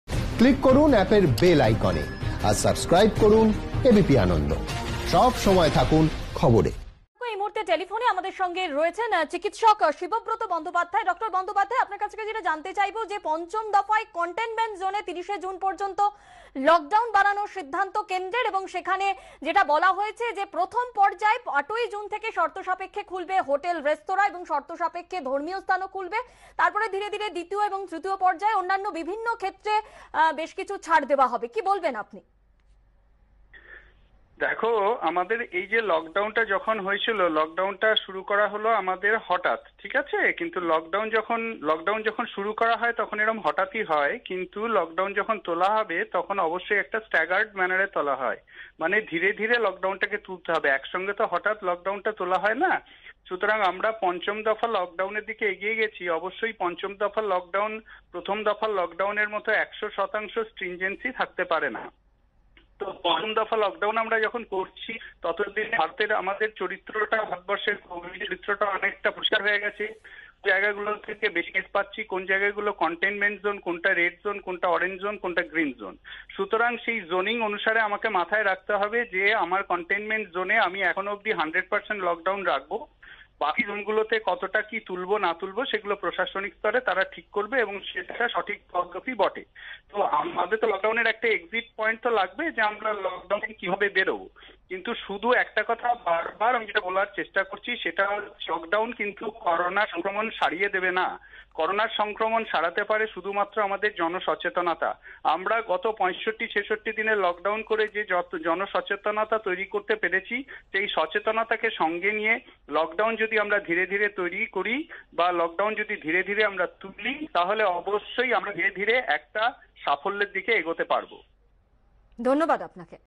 লকডাউন নয়, সচেতনতাই বাঁচাতে পারে করোনা থেকে, মত চিকিৎসক শিবব্রত বন্দোপাধ্যায়ের
পঞ্চম দফার লকডাউনে শর্তসাপেক্ষে ছাড় অনেক ক্ষেত্রেই। এপ্রসঙ্গে চিকিৎসক শিবব্রত বন্দোপাধ্যায় বলেন, "করোনা থেকে আমাদের বাঁচাতে পারে একমাত্র জনসচেতনতা।" তিনি আরও জানান, পঞ্চম অধ্যায়ের লকডাউন অবশ্যই প্রথম অধ্যায়ের লকডাউনের মতো হবে না, এবং লকডাউন তুলতে প্রয়োজন একটি সুনির্দিষ্ট পদ্ধতি।